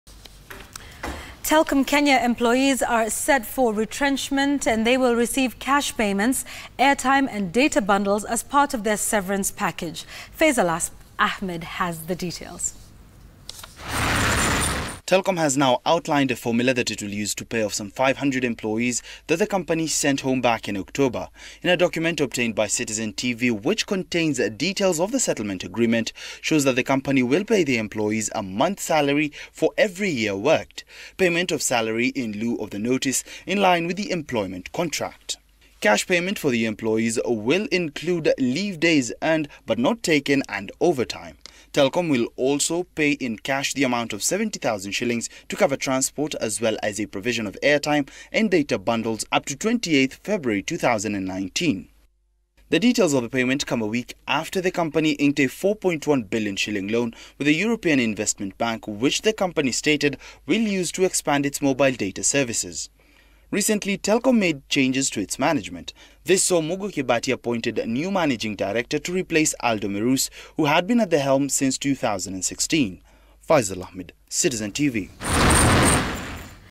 TELKOM Kenya layoffs
Telkom Kenya employees set for retrenchment will receive cash payments, airtime and data bundles as part of their severance package.